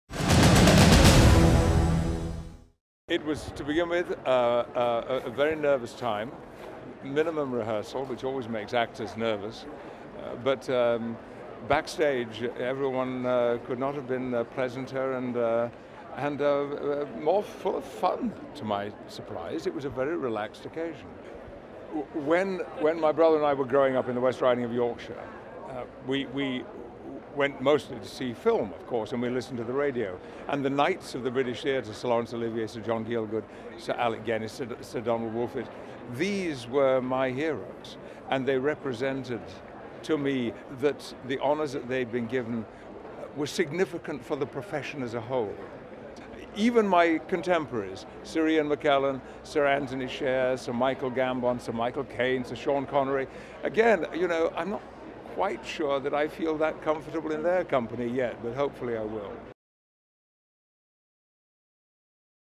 British acting legend Patrick Stewart knighted
The TV, film and theatre star recounts the moments before he was made a 'Sir' by the Queen.